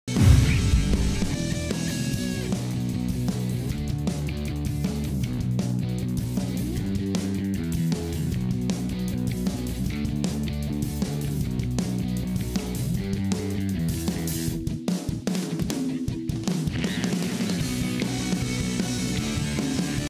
Panther of Week, UNI Wrestling's Parker Keckeisen - Big 12 Conference Wrestler of the Week
This weeks Panther of the Week is a UNI Wrestling's Parker Keckeisen after being named Big 12 Wrestler of the Week.

University of Northern Iowa, UNI Panthers